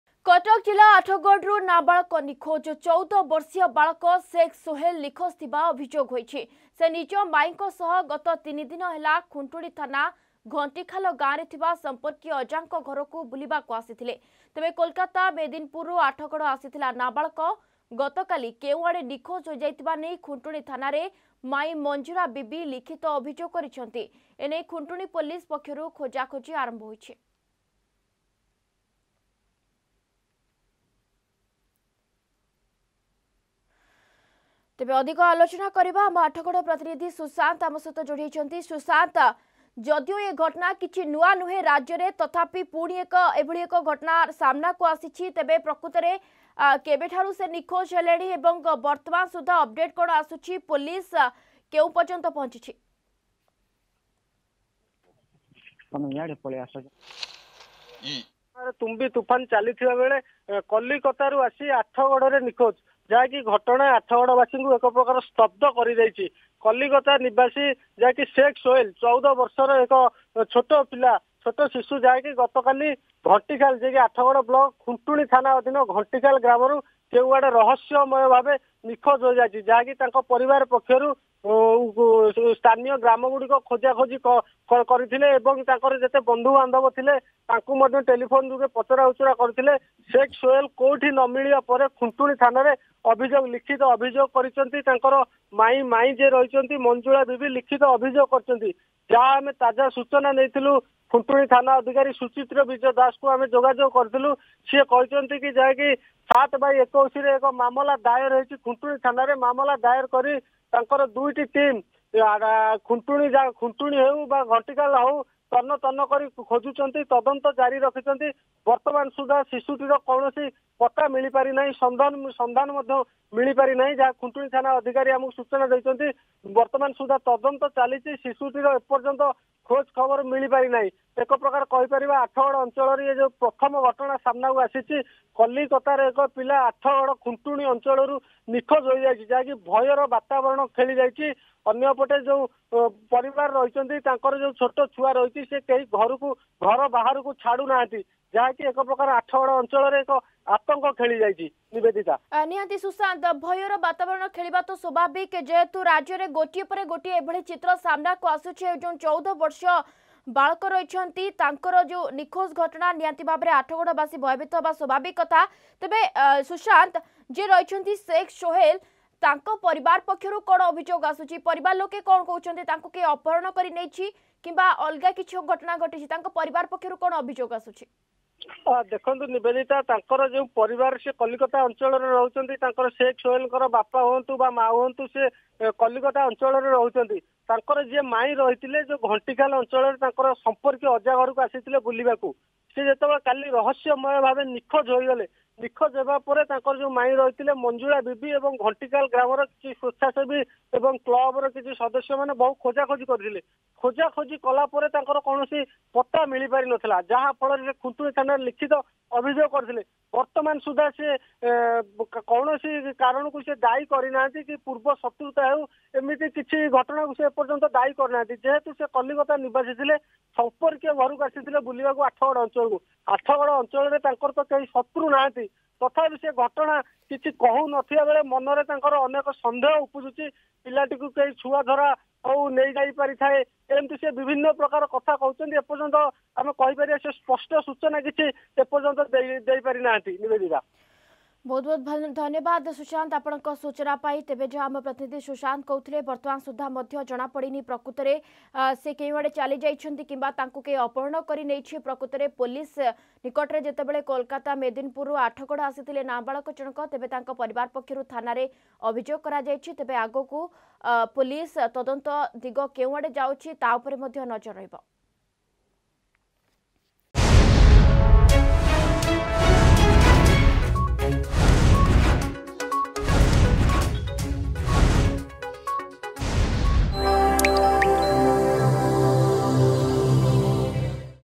କଟକ ଜିଲ୍ଲା ଆଠଗଡରୁ ନାବାଳକ ନିଖୋଜ .....
କଟକ ଜିଲ୍ଲା ଆଠଗଡରୁ ନାବାଳକ ନିଖୋଜ । 14 ବର୍ଷୀୟ ବାଳକ ସେଖ ସୋଏଲ ନିଖୋଜ ଥିବା ଅଭିଯୋଗ ହୋଇଛି । ସେ ନିଜ ମାଇଁଙ୍କ ସହ ଗତ 3 ଦିନ ହେଲା ଖୁଣ୍ଟୁଣୀ ଥାନା ଘଣ୍ଟିଖାଲ ଗାଁରେ ଥିବା ସମ୍ପର୍କୀୟ ଅଜା ଘରକୁ ବୁଲିବାକୁ ଆସିଥିଲା .